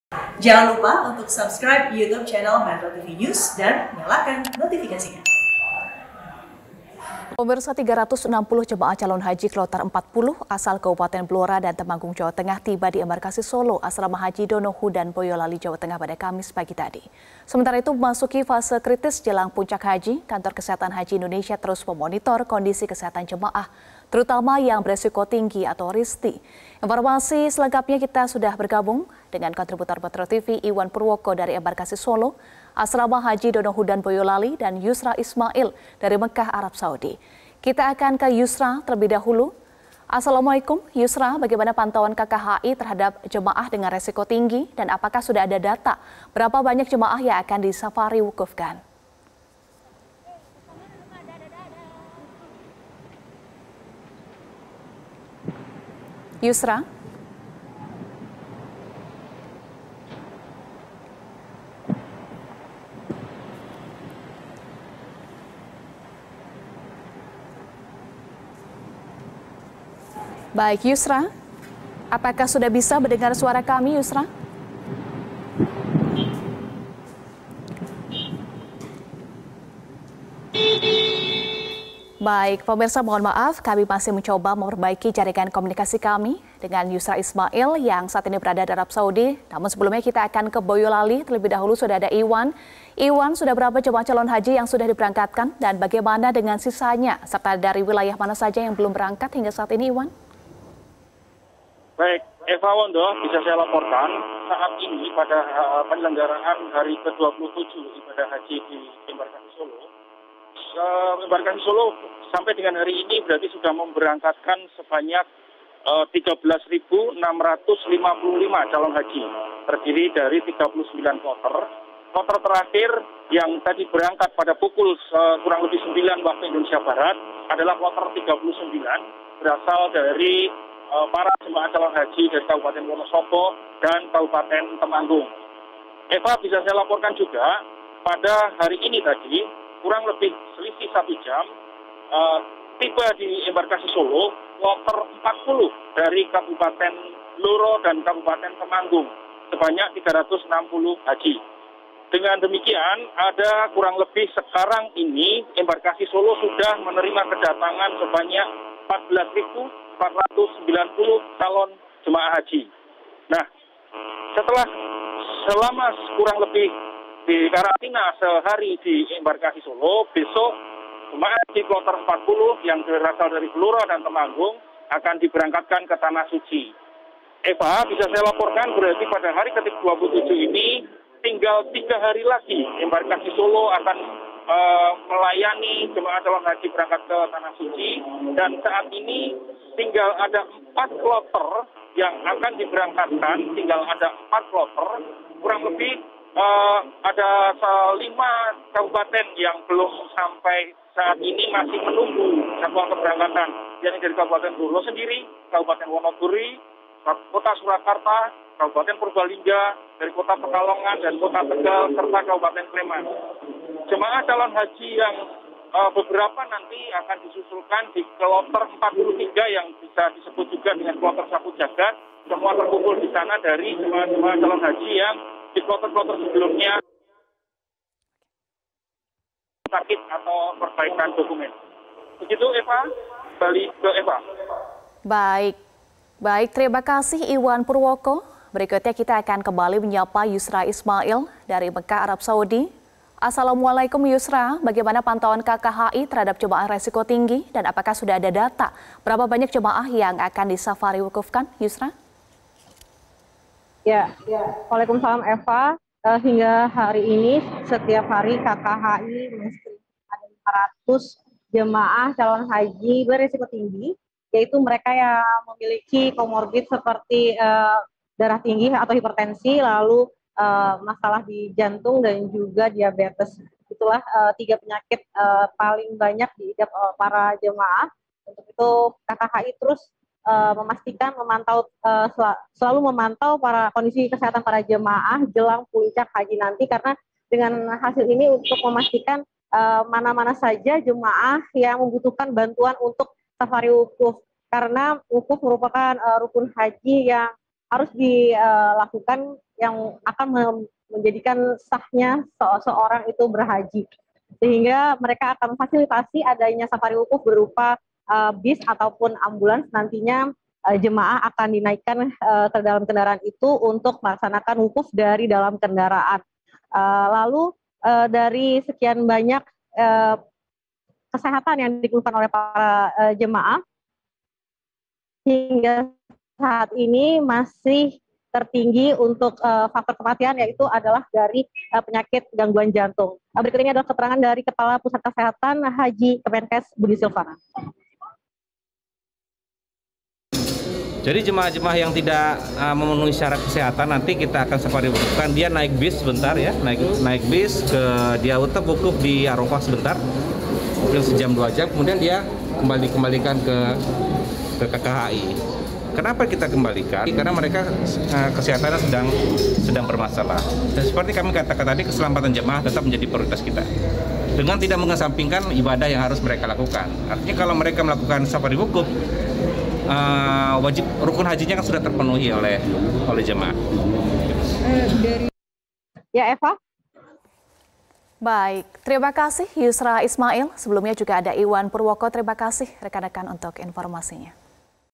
KKIH Monitor Kesehatan Jemaah Haji untuk Menentukan Safari Wukuf
Sebanyak 360 calon haji kloter 40 asal Kabupaten Blora dan Temanggung Jateng tiba di Embarkasi Solo Asrama Haji Donohudan pada Kamis (30/06/2022) pagi.

Sementara itu, memasuki fase kritis puncak haji, KKIH terus memonitor kondisi kesehatan jemaah, terutama yang berisiko tinggi penyakit. Kondisi kesehatan ini nantinya akan menentukan apakah jemaah safari wukuf atau tidak. Safari wukuf ini diperuntukkan untuk jemaah haji yang sedang sakit dan dirawat di KKIH Mekkah.